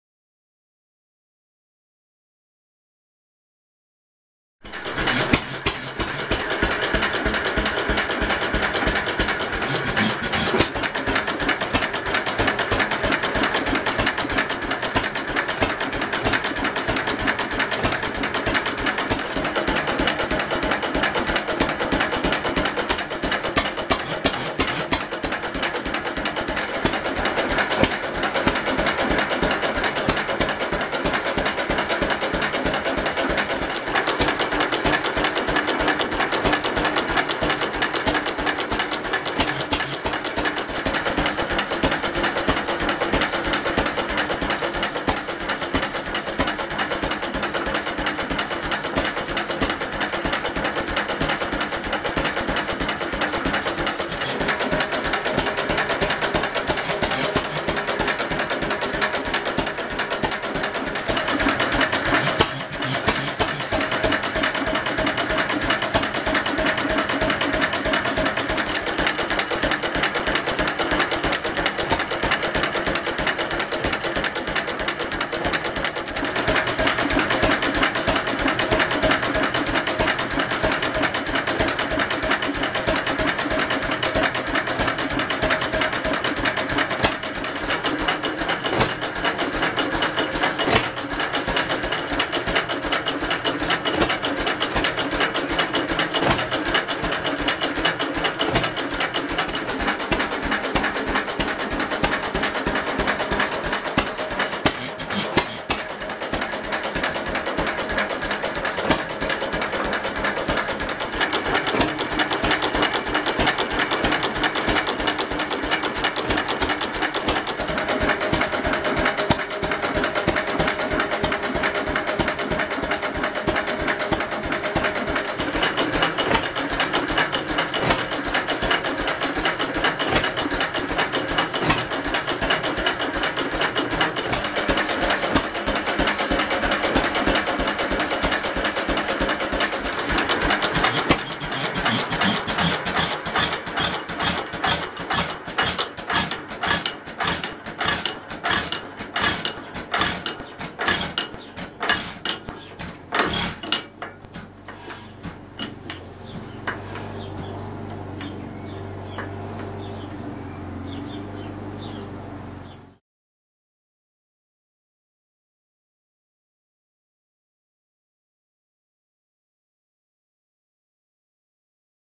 Fairbanks Morse 3hp Stationary Engine
This is a short movie filmed on my Canon 5D MkII by my partner. The first time we have compiled a HD movie on a pro DSLR camera and uploaded it to the web.